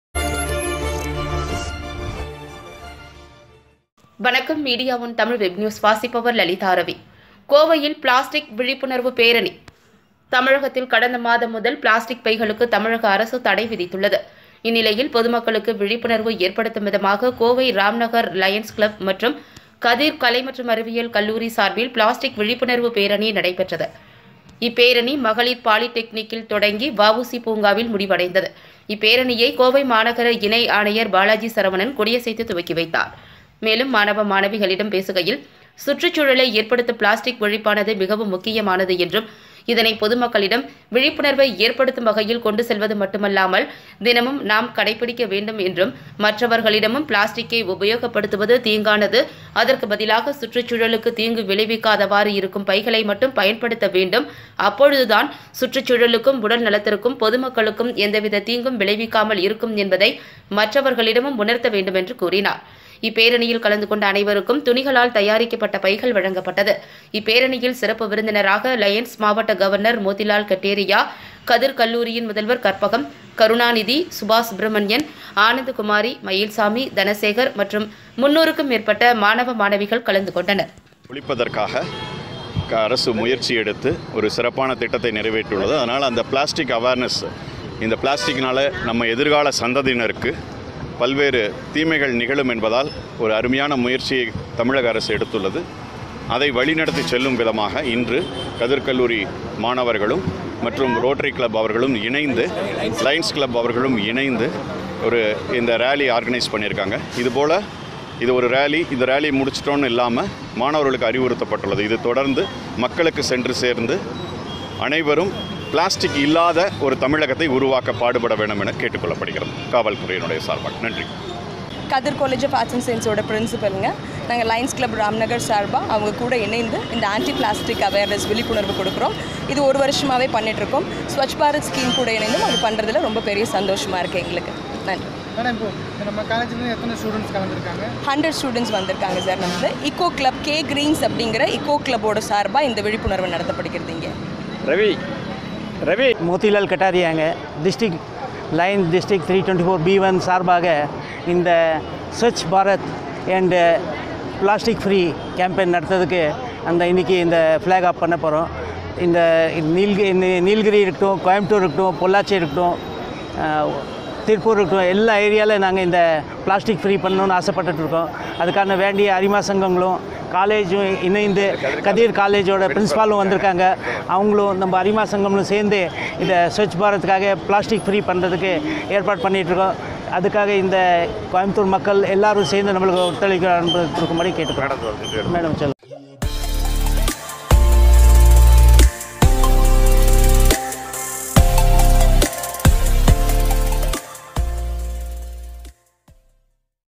பொது மக்களுக்கு விழிப்புணர்வு ஏற்படுத்தும் விதமாக பேரணி
தமிழகத்தில் கடந்த மாதம் முதல் பிளாஸ்டிக் பைகளுக்கு தமிழக அரசு தடைவிதித்துள்ளது மேலும் பொது மக்களுக்கு விழிப்புணர்வு ஏற்படுத்தும் விதமாக கோவையில் பேரணி நடைபெற்றது.

கோவை ராம்நகர் லயன்ஸ் கிளப் மற்றும் கதிர் கலை மற்றும் அறிவியல் கல்லூரியும் இணைந்து பிளாஸ்டிக் விழிப்புணர்வு பேரணி நடைப்பெற்றது. இப்பேரணி மகளிர் பாலிடெக்னிக் தொடங்கி வ. உ .சி பூங்காவில் முடிவடைந்தது. இப்பேரணியை கோவை மாநகர இணை ஆணையர் பாலாஜிசரவணன் கொடியசைத்து துவக்கி வைத்தார். மேலும் மாணவ மாணவிகளிடம் பேசுகையில் சுற்றுச் சுழலை ஏற்படுத்தும் பிளாஸ்டிக் ஒழிப்பானது மிகவும் முக்கியமானது, இதனை பொதுமக்களிடம் விழிப்புணர்வை ஏற்படுத்தும் வகையில் கொண்டு செல்வது மட்டுமல்லாமல் தினமும் நாம் கடைபிடிக்க வேண்டும். மற்றவர்களிடமும் பிளாஸ்டிக்கை உபயோகபடுத்துவது தீங்கானது என்றும்  எப்பொழுதும் பயன்படுத்தவேண்டாம் அதற்கு பதிலாக சுற்றுச்சூழலுக்கு தீங்கு விளைவிக்காதவாறு இருக்கும் பைகளை மட்டும் பயன்படுத்த வேண்டும் அப்பொழுதுதான் சுற்றுச்சூழலுக்கும், உடல்நலத்திற்கும், பொதுமக்களுக்கு எந்தவித தீங்கும் விளைவிக்காமல் இருக்கும் என்பதை உணர்த்த வேண்டும் என்று கூறினர். மற்றும் இப்பேரணியில் கலந்து கொண்ட அனைவருக்கும் துணிகளால் தயாரிக்கப்பட்ட பைகள் வழங்கியது. இப்பேரணியில் சிறப்பு விருந்தினராக லயன்ஸ் மாவட்ட கவர்னர் மோதிலால்கட்டேரியா, கதிர் கல்லூரியின் முதல்வர் கற்பகம், கருணாநிதி,  சுபாசுப்பிரமணியன், ஆனந்தகுமரி, மயில்சாமி, தனசேகர் 300-க்கும் மேற்பட்ட மாணவ மாணவிகள் கலந்து கொண்டனர்.

CATEGORY -NEWS/POLITICS